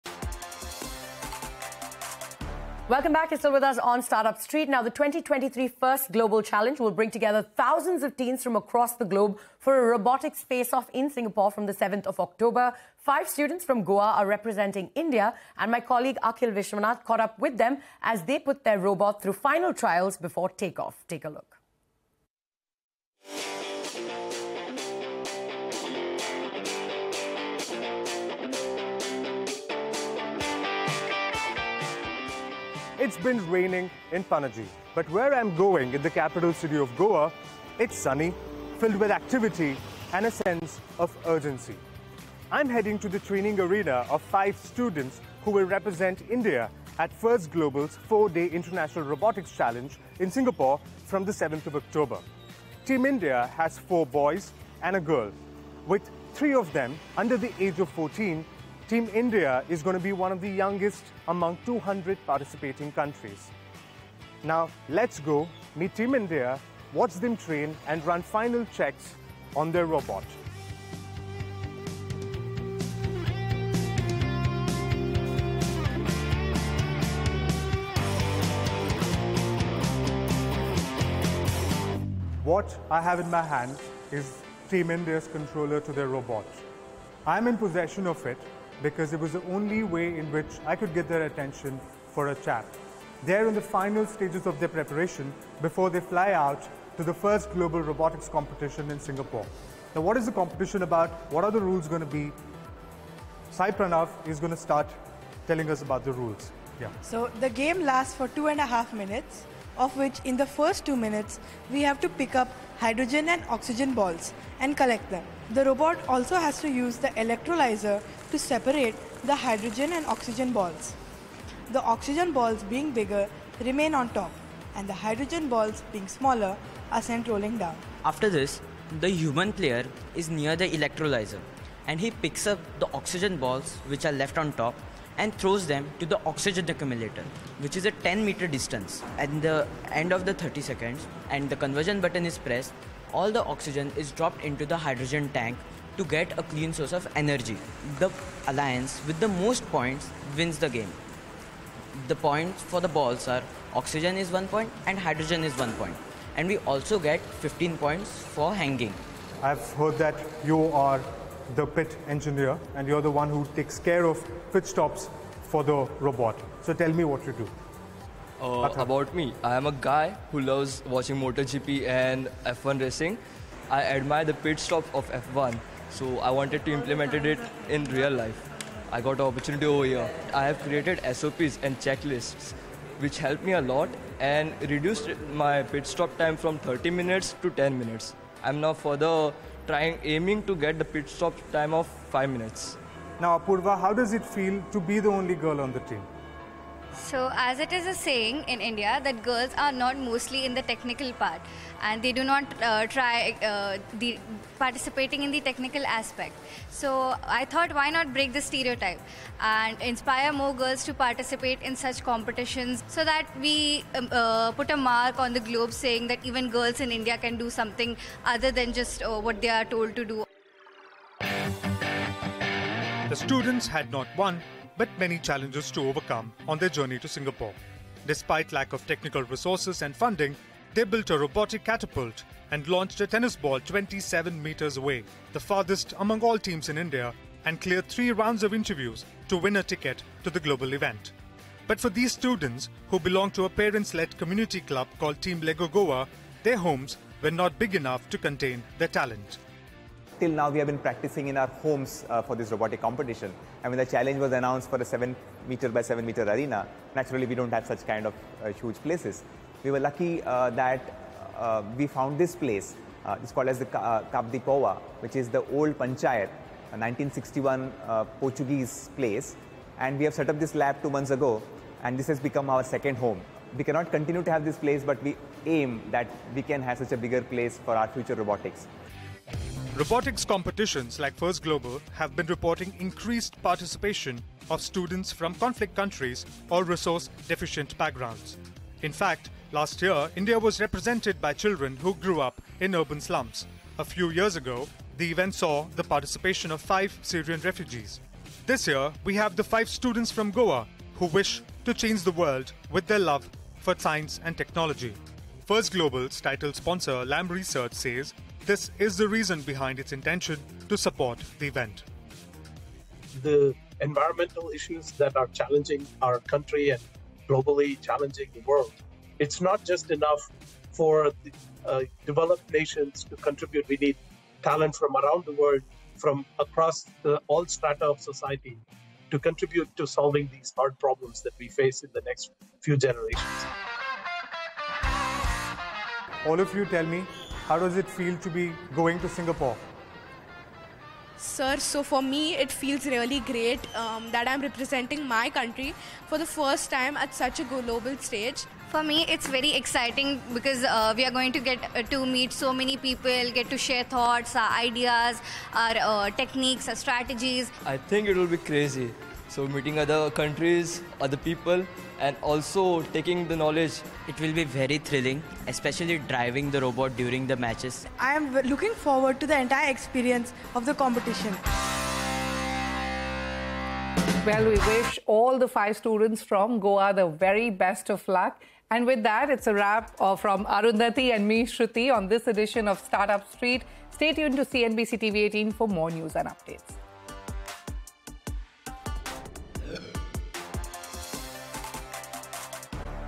Meet The 5 Students Representing India At 'The FIRST Global Challenge' Competition' | CNBC TV18
FIRST Global uses robots to build kids with the self-confidence, skill sets, imagination, courage, and vision to do essential things. Tune in for more details.

n18oc_business